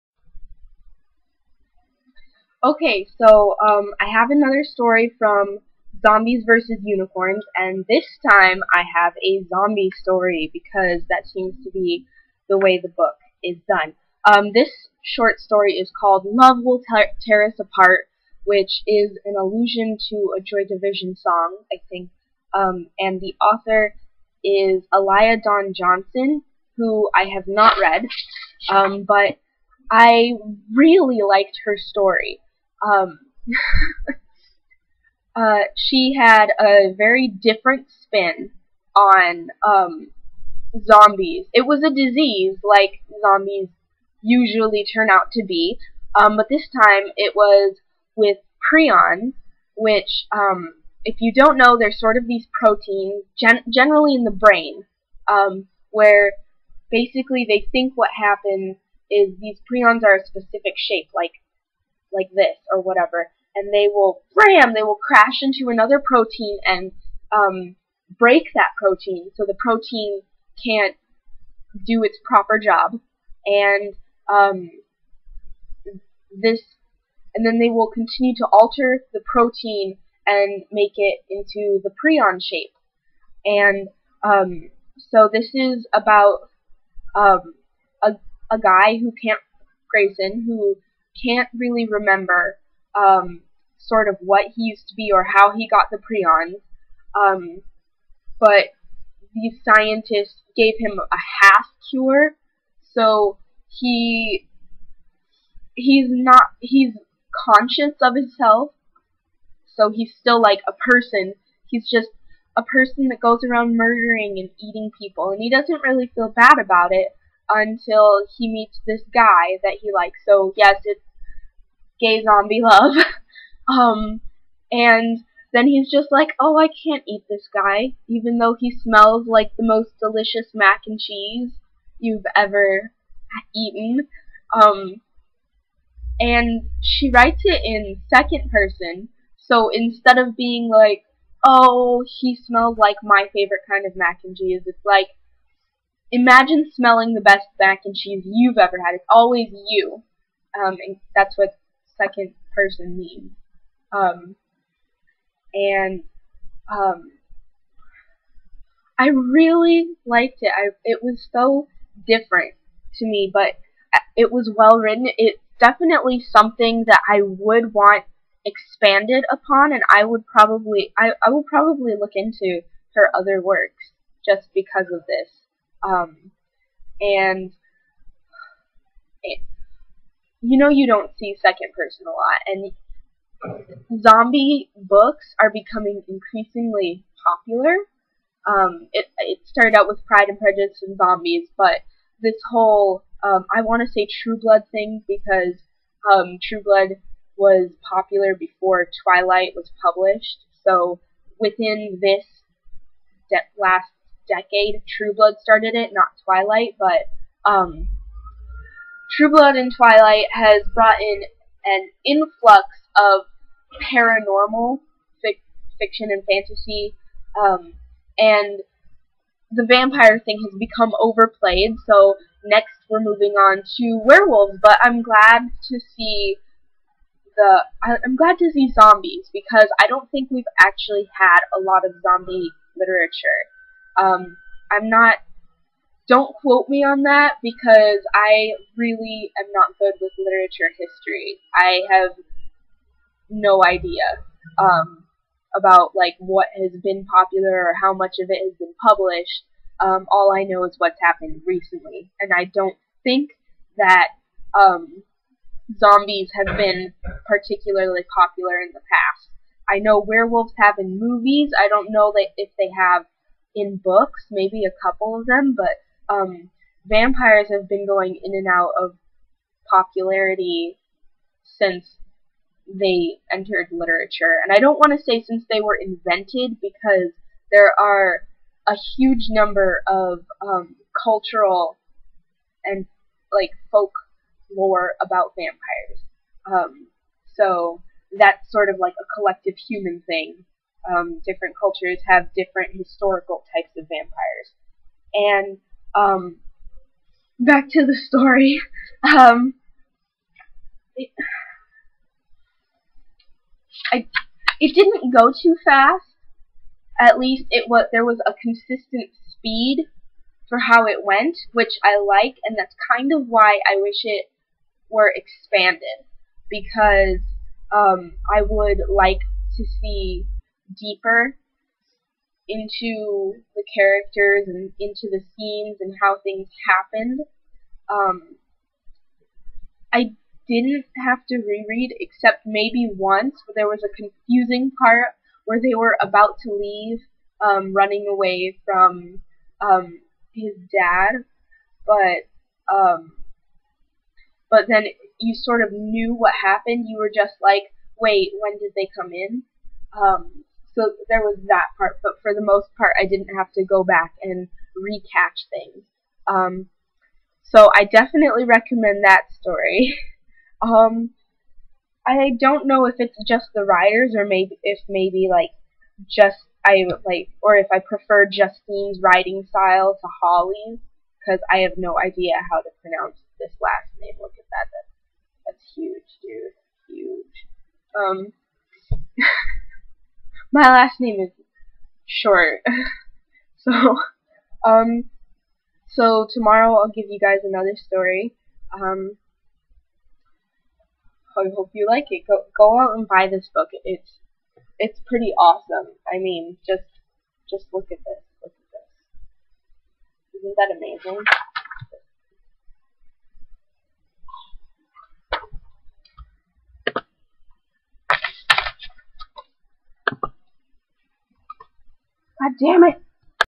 3 Zombies vs Unicorns, Alaya Dawn Johnson, Love Will Tear Us Apart
This is my review of a short story in an anthology edited by Holly Black and Justine Larabalestier, published in 2010.